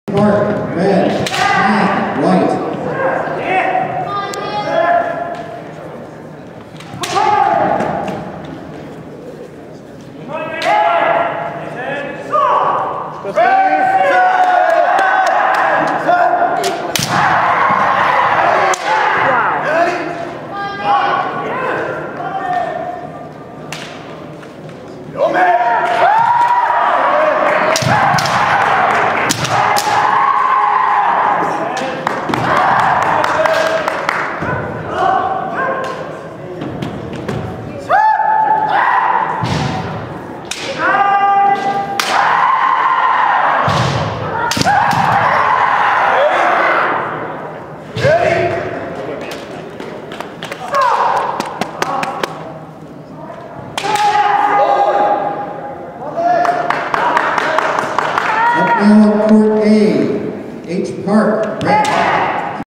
2011 Rose Festival Taikai - Mens 3 Kyu - Steven Le (Idaho) vs McRae (Spokane)
Steven Le from Idaho Kendo Club against McRae from Spokane Kendo Club at the 2011 Rose Festival Taikai.